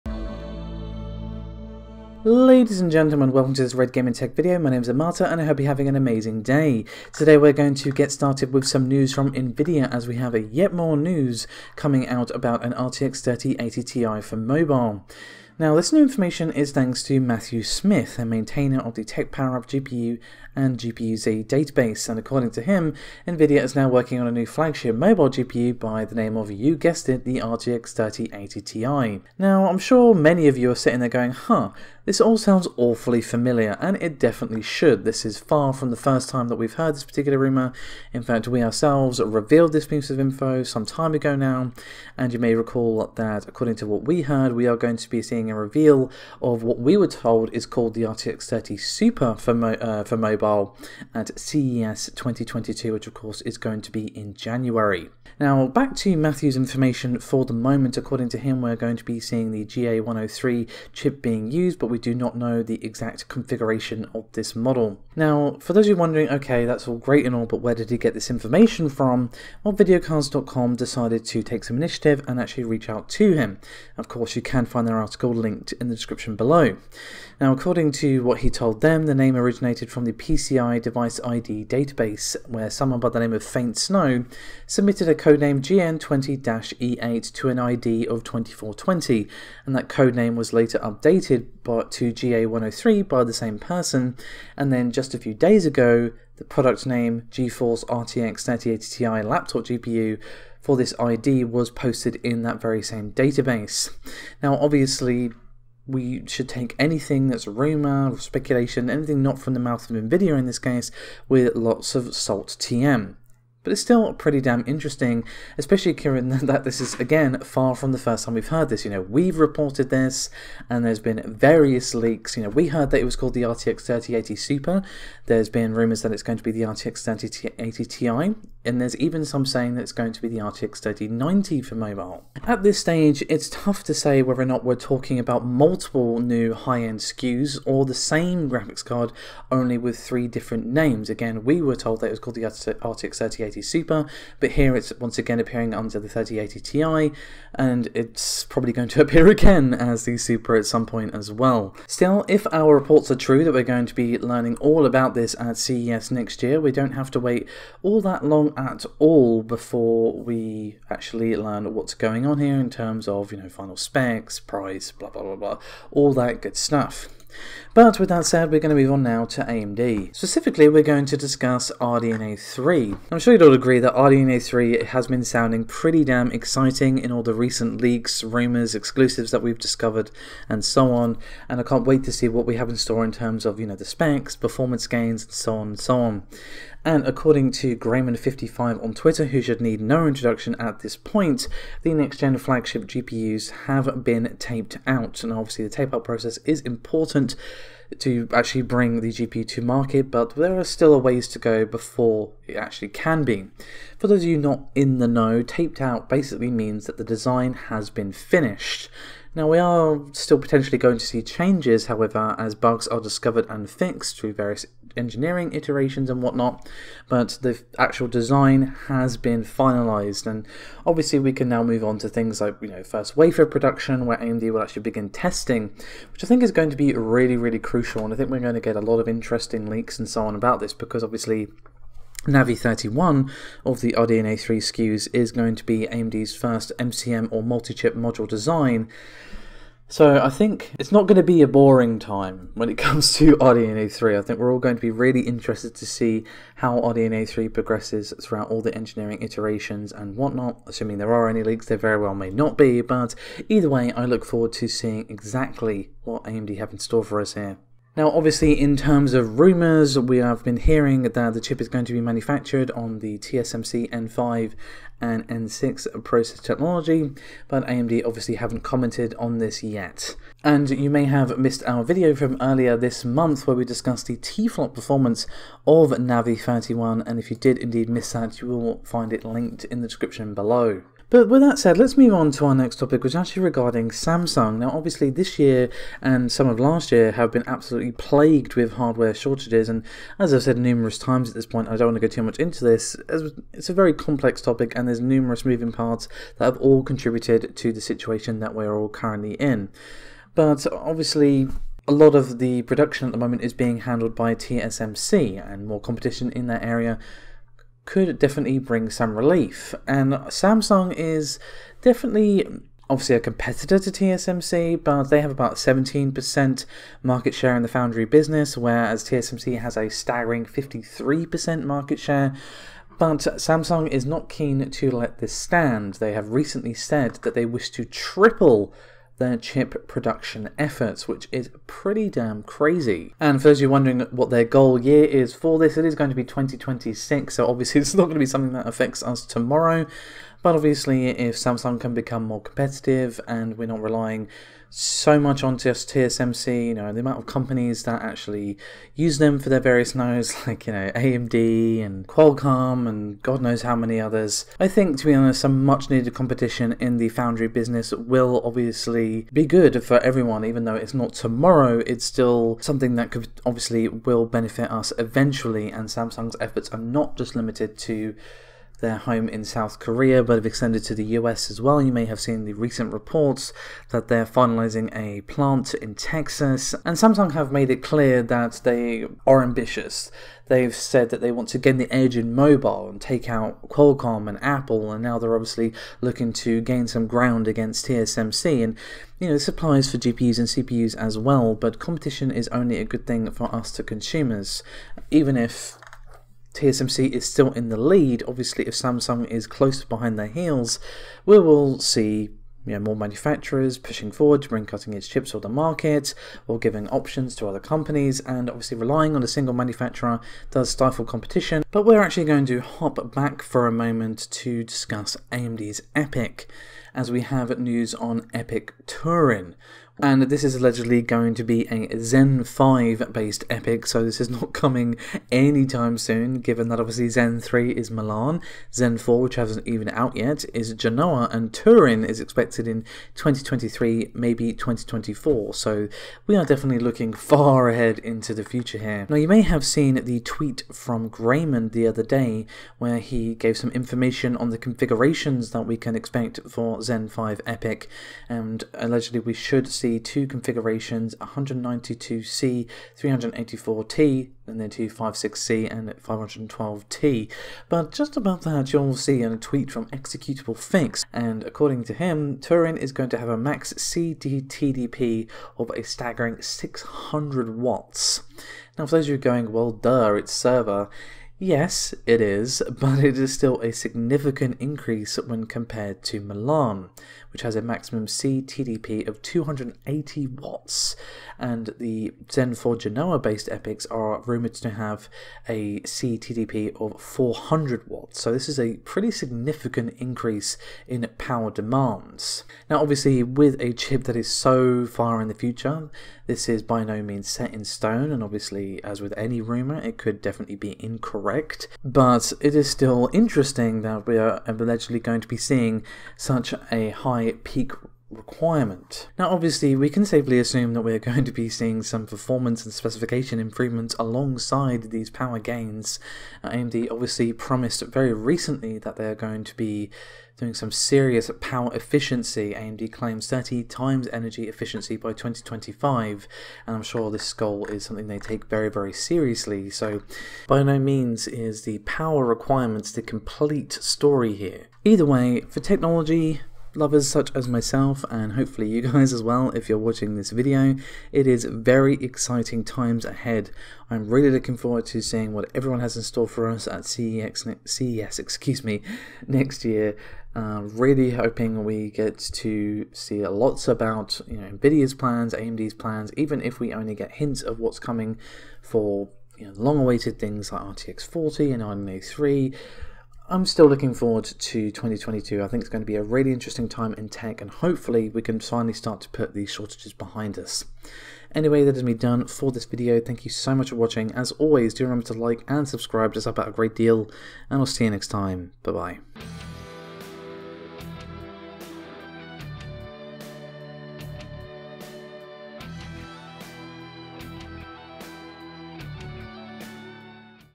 RDNA 3 Is COMING - AMD's Next Gen Just Took a Major Step Forward | RTX 3080 Ti Mobile Details LEAK
AMD's RDNA 3 is hugely anticipated, as the Navi 31 SKUs will be their first CONSUMER MCM GPU design. The recent leaks and rumours surrounding the architecture, hardware specs and performance improvements for gaming and ray tracing are very impressive indeed, so anticipation is high to see how RDNA 3 RX 7000 graphics cards fare vs RTX 40 Lovelace GPUs from Nvidia. AMD RDNA 3 just took a MAJOR step forward, as the next gen of AMD graphics cards just took a MAJOR step forward. But will the specifications, and gaming and ray tracing performance benchmarks impress compared to Nvidia Lovelace RTX 40 GPUs?

We also have info on RTX 3080 Ti mobile / RTX 3080 SUPER for laptops, as more details have leaked on the long rumoured RTX 30 SUPER GPUs for mobile. But with reports also claiming that we will get the RTX 30 SUPER on both desktop AND mobile, will the GA103 chip with the hardware specs and performance improvements be enough to make the RTX 3080 SUPER / RTX 3080 Ti laptop graphics cards worth it for gamers, especially amongst the shortages?

0:00 Start
0:09 RTX 3080 Ti mobile
2:50 RDNA 3
4:40 Samsung production 3x
6:59 EPYC Turin